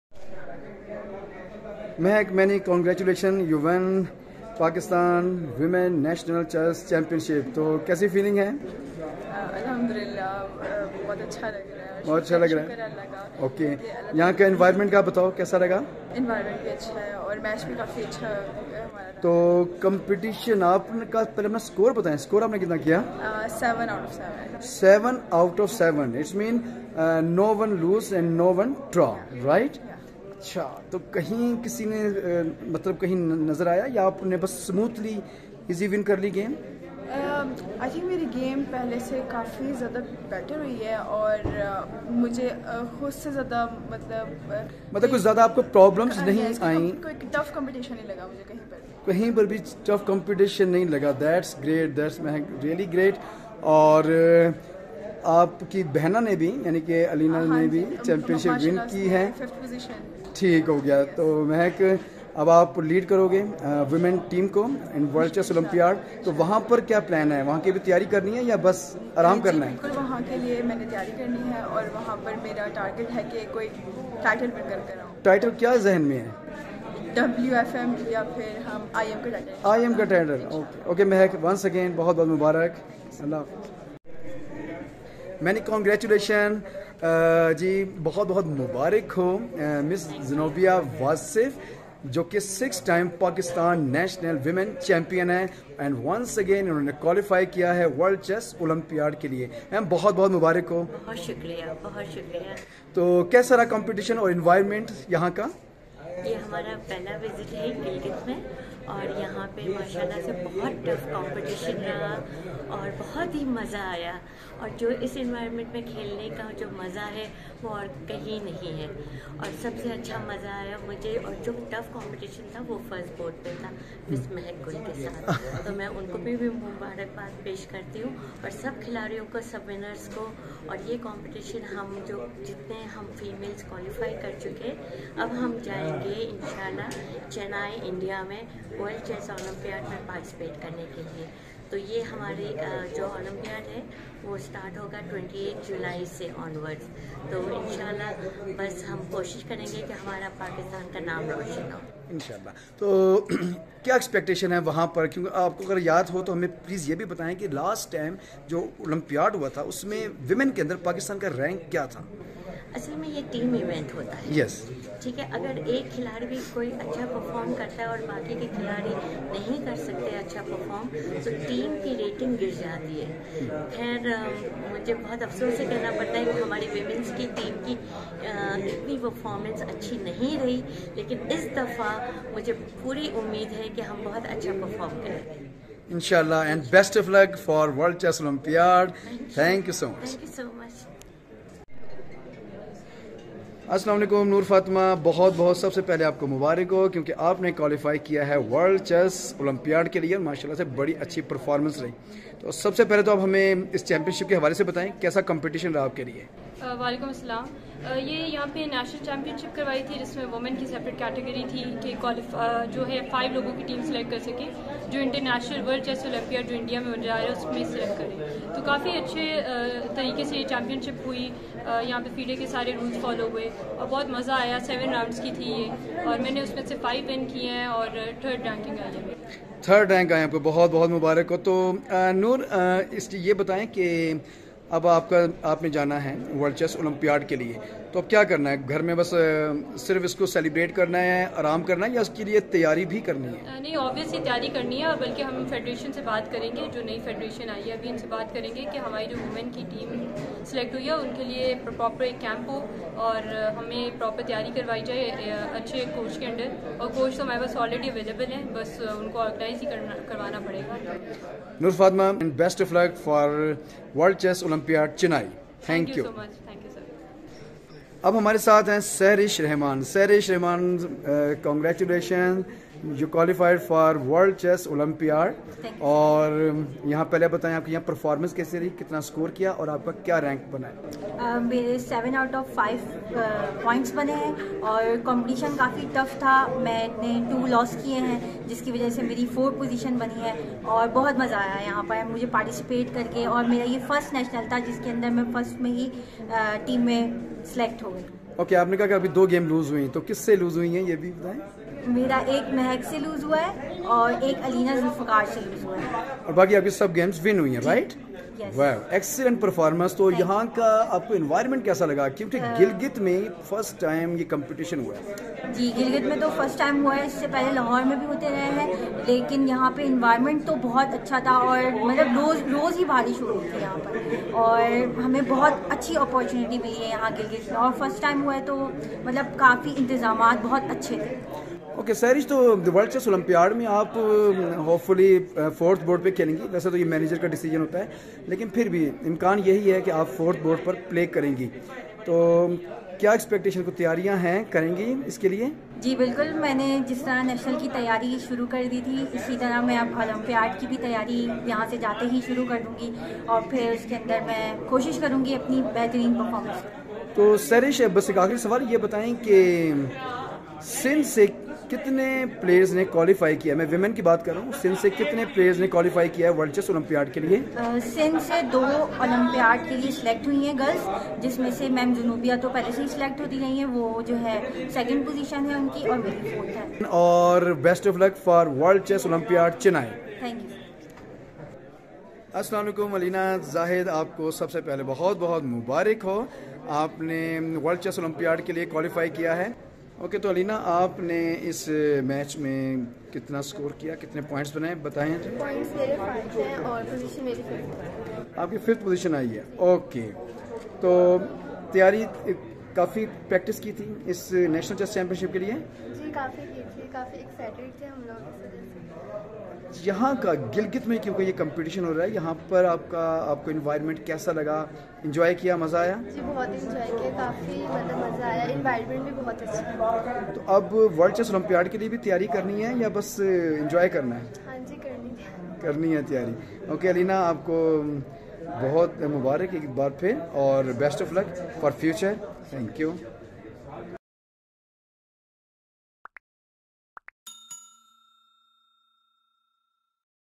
TOP 5 Pakistani Women Chess Players Who Qualify for World Chess Olympiad 2022 India Chennai
Pakistan National Men/Women  Chess Championship 2022 Gilgit Baldistan.
An short interview with TOP 5 Pakistani Women Chess Players Who Qualify for World Chess Olympiad 2022 India Chennai.